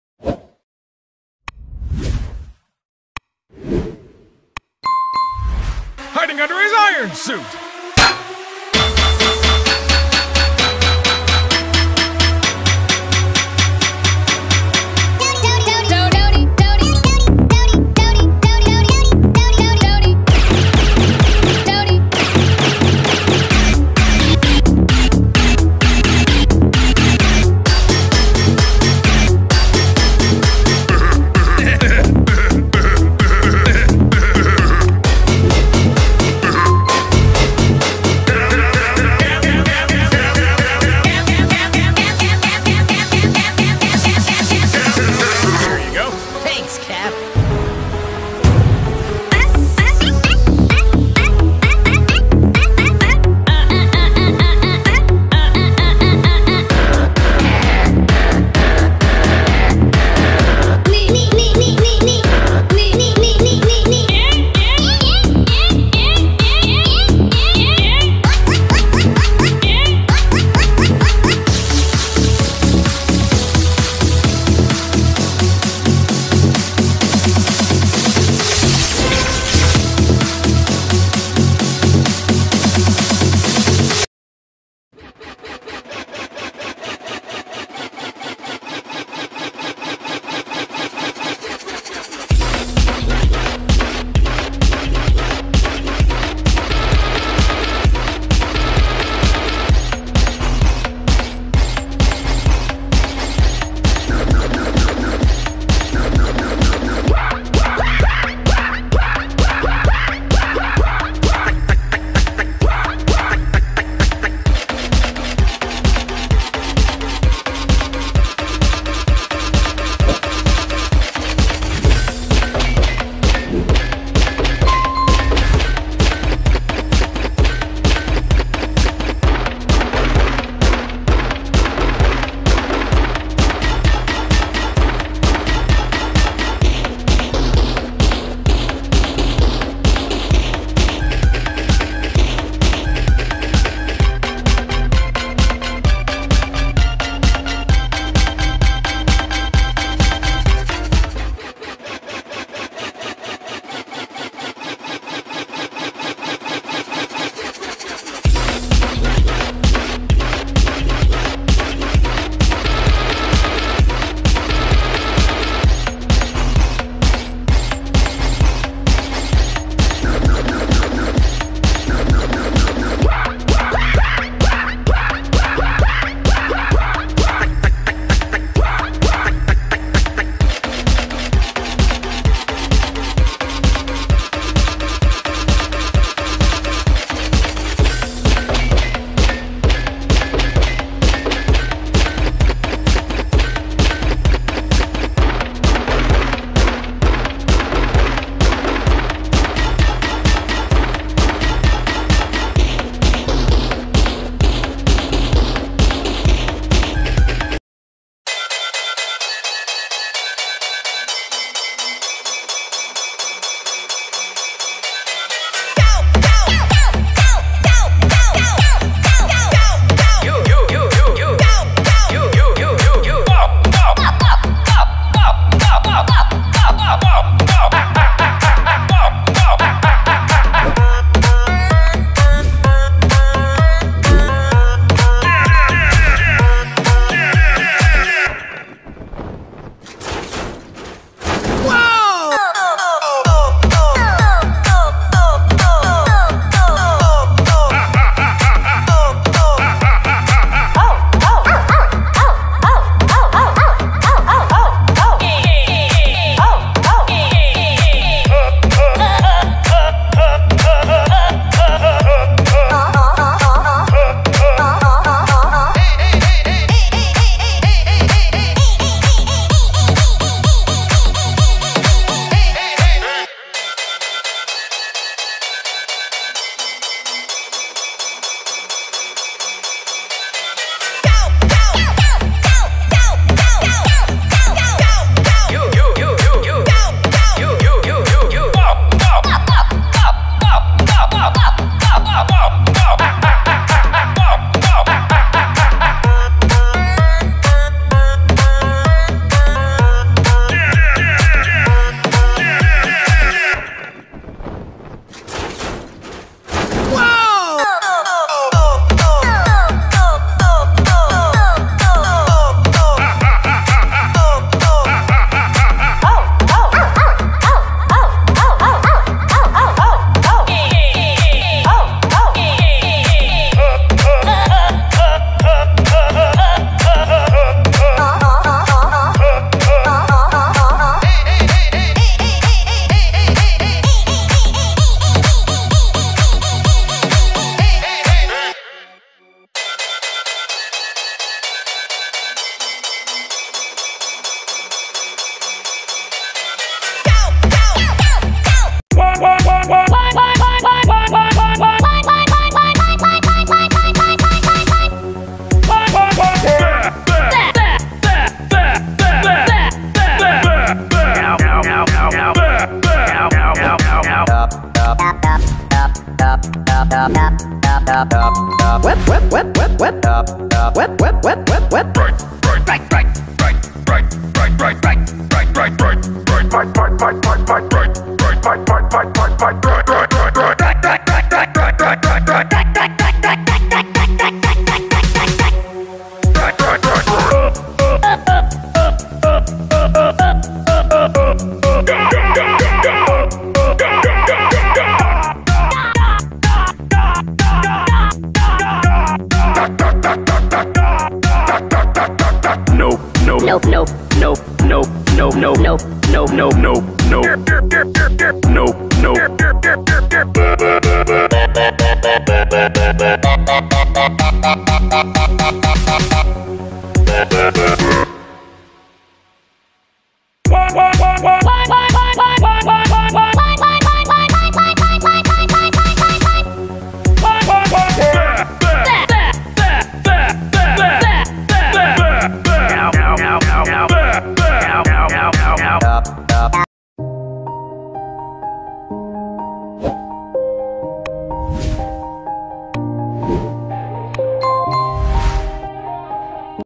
Telis Hop EDM & Phonk Rush - Hulk vs Captain America vs Spider-Man vs Thanos
Your favorite superheroes are featured in this video. Spider-Man, the Hulk and other interesting characters dance to fun music.
Expect updates, because each time you will find more and more interesting content.